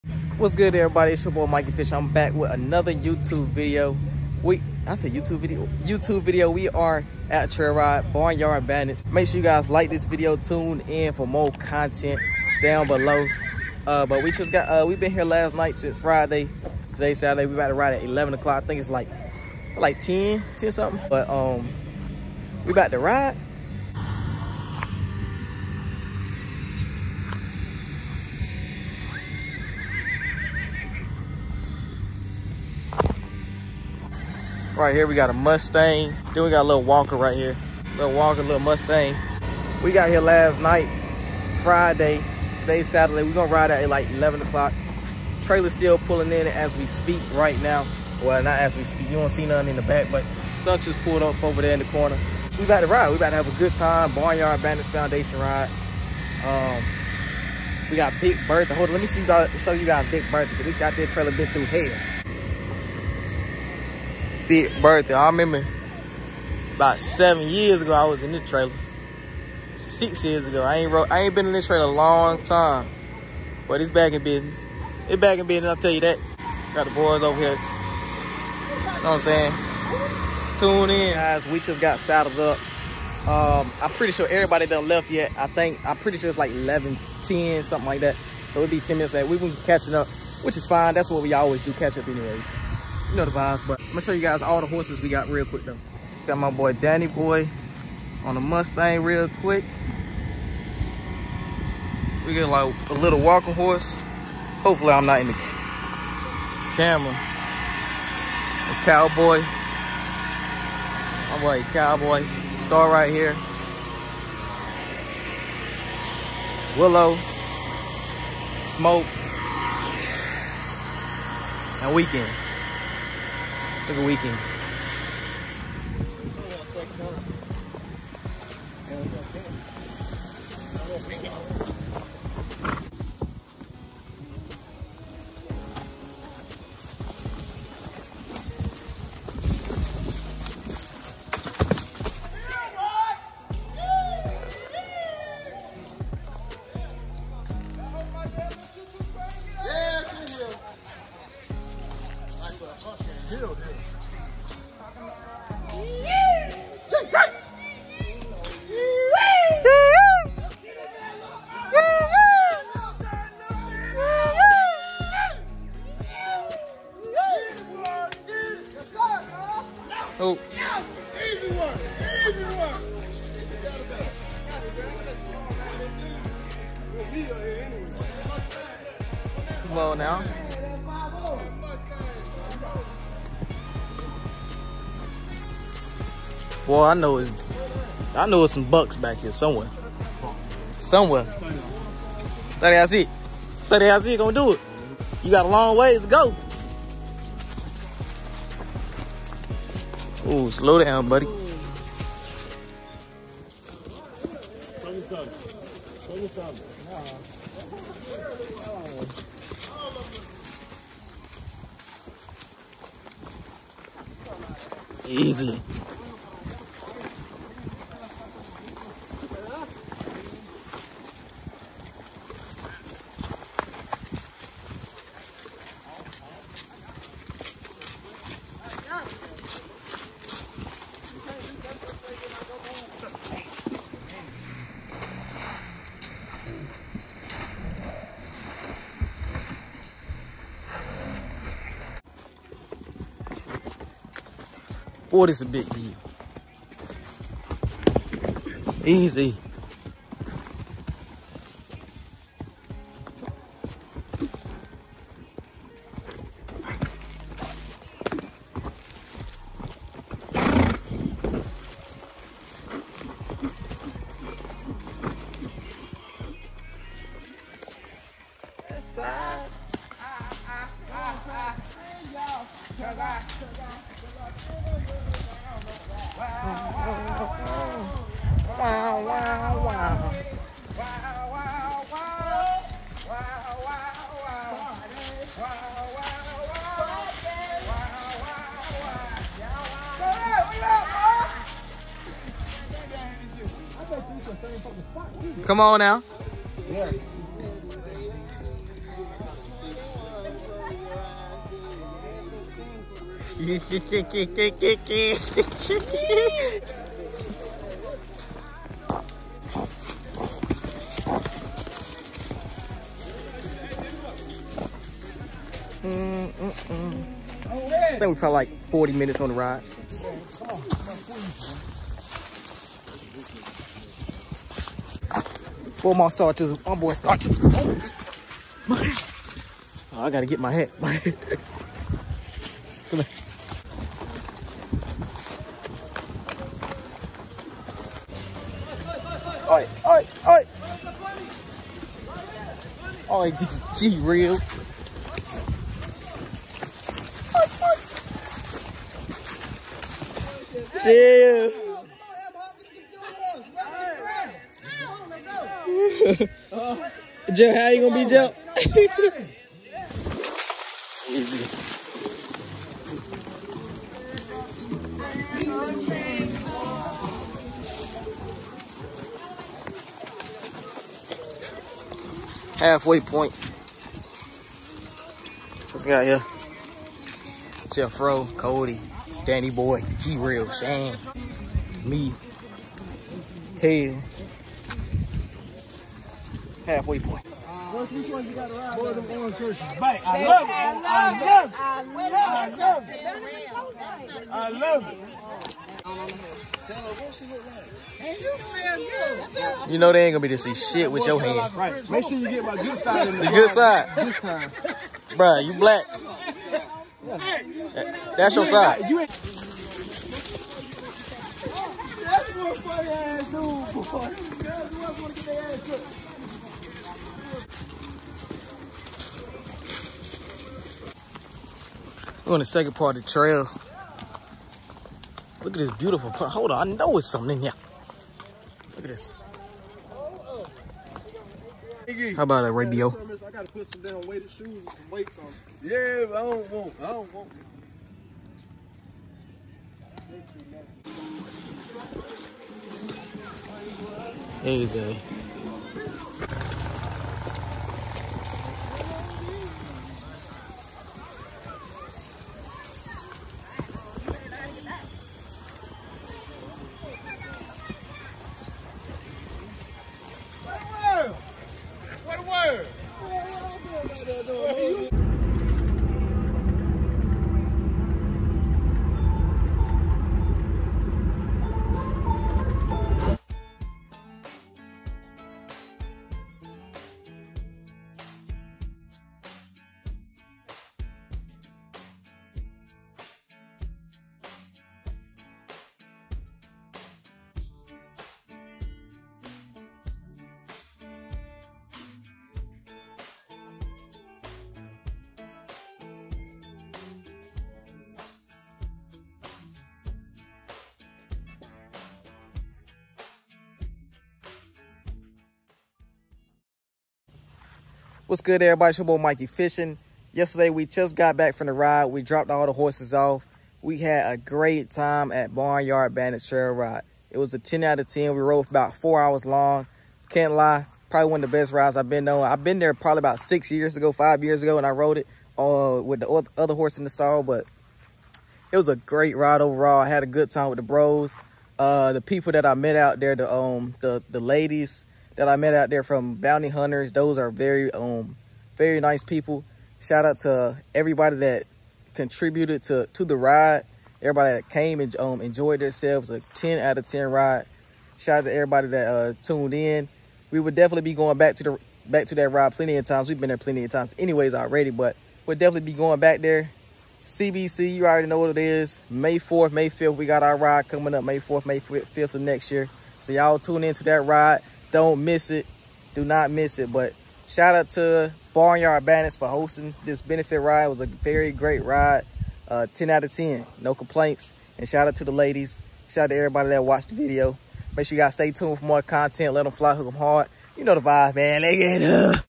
ONE Of The SMOOTHEST Trail Rides in Nc | (Barn Yard Bandits Benefit Ride)
smooth, not to complicated but just enough to give your horse a good workout typa Ride! Barn Yard Bandits Thankyou 🤝🏽🏇

🚨 beacons.ai/mikeyfishing 🚨

🎣 Let Em Fly, and Hookem Hard 🎣